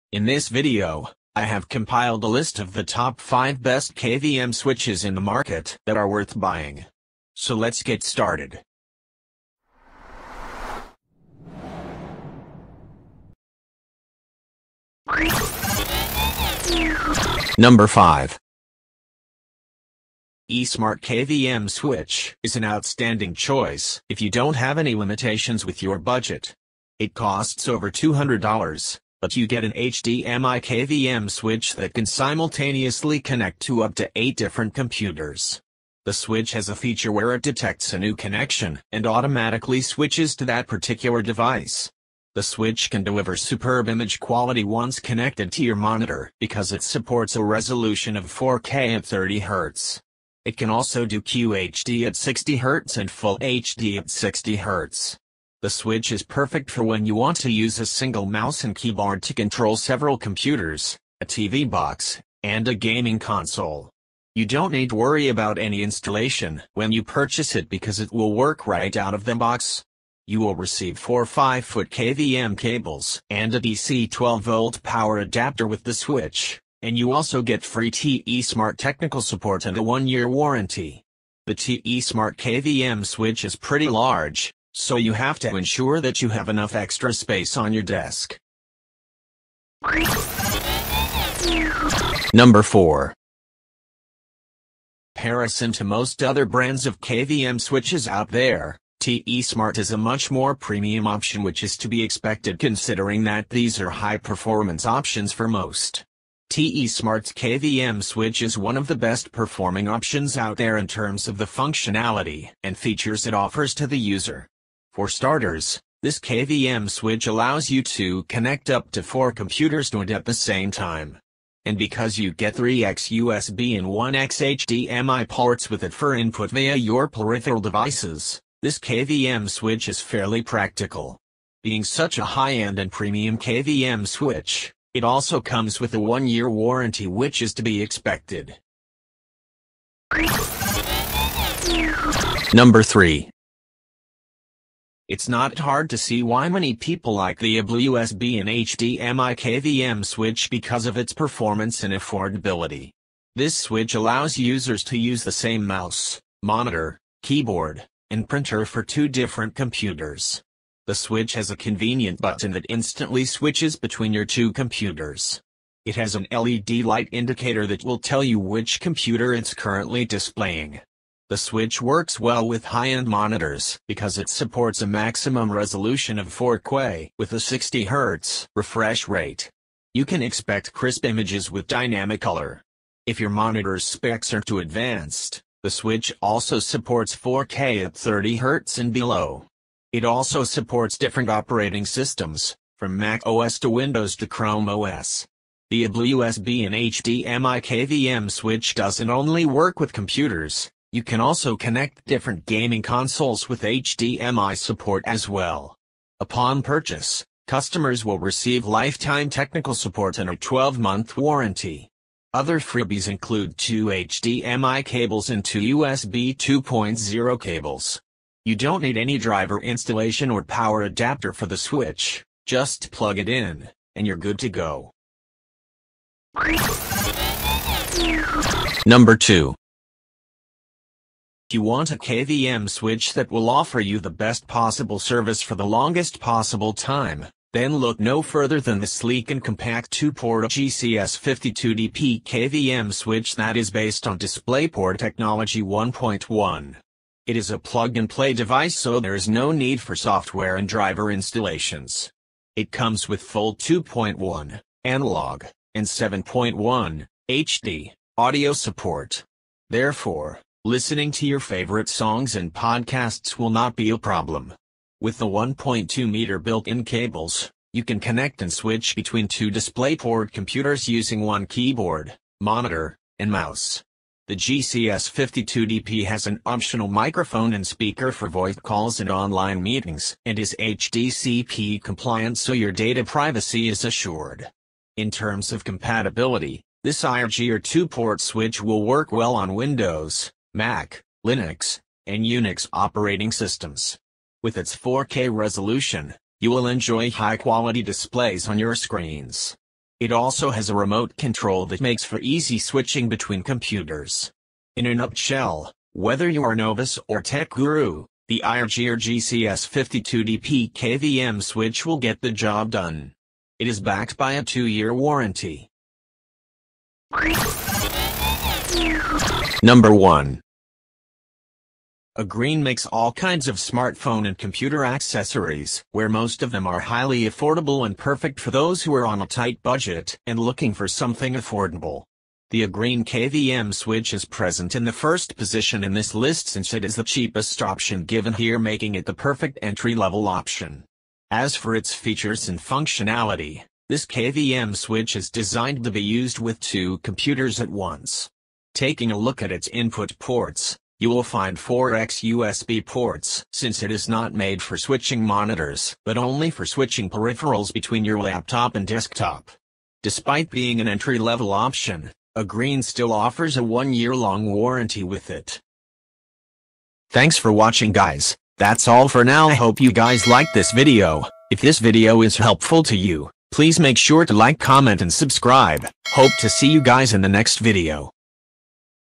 Top 5 Best KVM Switches Reviews of 2024
In This Video, I Listed "Top 5 Best KVM Switches 2024" You Can Check the discount Price or Purchase in the Description Below!

👉05.Unnlink 4K HDMI KVM Switch

👉04.UGREEN 10Gbps USB C KVM Switch

👉03.KVM Switch Dual Monitor DisplayPort 1.4 8K@30Hz 4K@144Hz 2 in 2 Out,DP1.4 KVM Switch

👉02.Dual Monitor KVM Switch USB 3.0 HDMI KVM Switch

👉01.2024 NEW KCEVE HDMI USB 3.0 KVM Switch